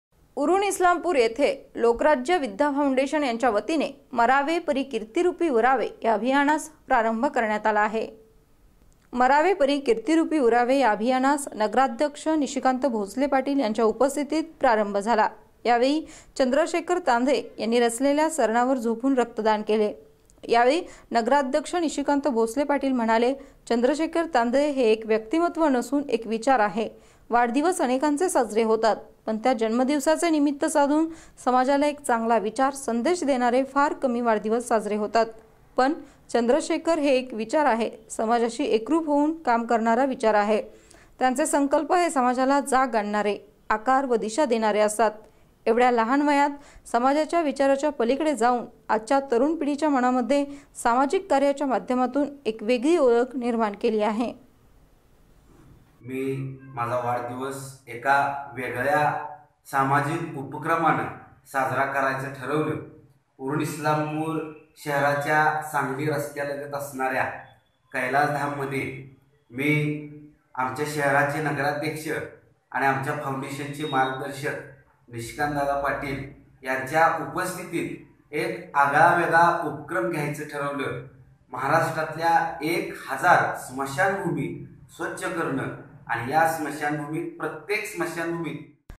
सांगली : -इस्लामपूर येथील विद्या फौंडेशनच्या वतिने "मरावे परि किर्ती रूपी उरावे,अभियानास प्रारंभ.
सांगली : - उरुण-इस्लामपूर येथील लोकराज्य विद्या फौंडेशनच्या वतिने "मरावे परि किर्ती रूपी उरावे,अभियानास प्रारंभ.पाहा फक्त SBN मराठी.